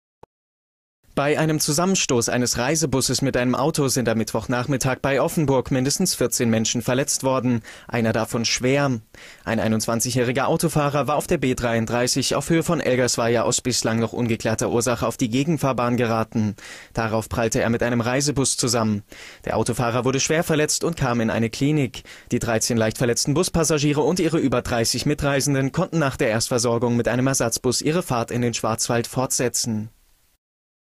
Offenburg - Frontalcrash mit Reisebus
Bei Offenburg sind mindestens 14 Menschen bei einem Zusammenstoß eines Reisebusses mit einem PKW verletzt worden.